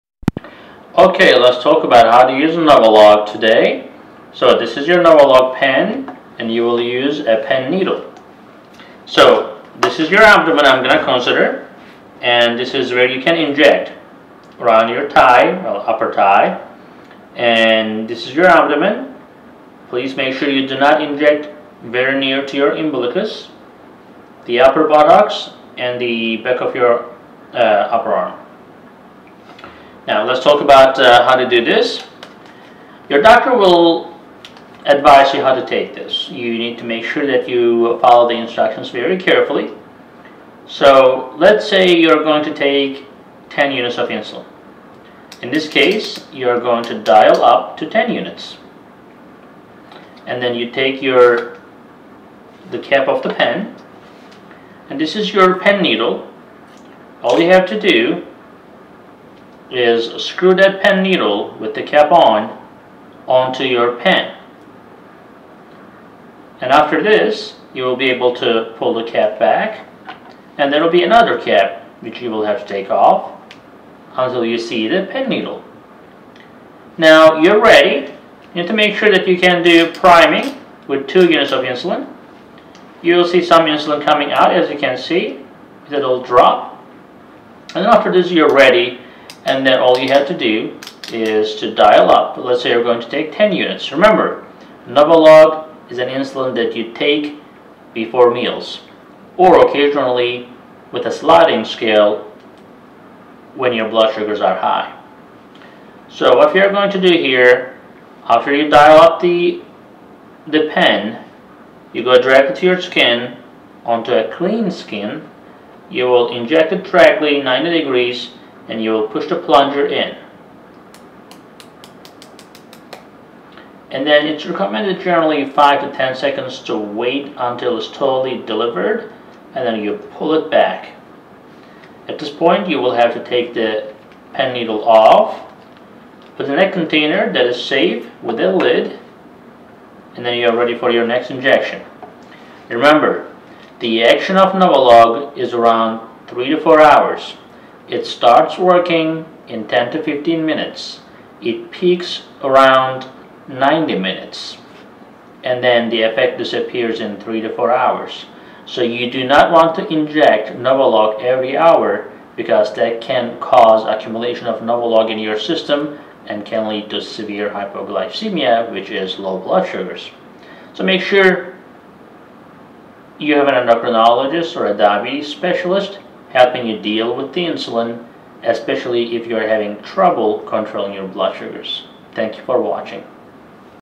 How To Use Novolog Flexpen? [Doctor Shows and Explains - 2020]
How to use NovoLog? Dr. Ergin, the founder, and CEO of sugarMDs.com will show and explain in this video how to use the Novolog Flexpen. Novolog should be administered by subcutaneous injection in the abdominal region, buttocks, thigh, or upper arm. Because NovoLog has a more rapid onset and a shorter duration of activity than human regular insulin, it should be injected immediately (within 5-10 minutes) before a meal.